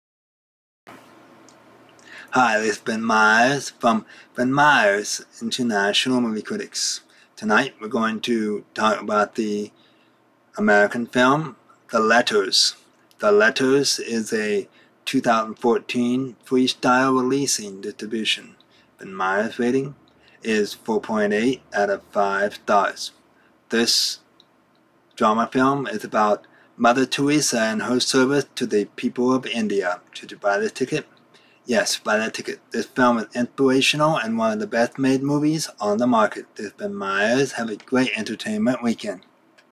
The Letters (2014)
Ben Meyers International Movie Critics offers a written critique of “The Letters” (2014)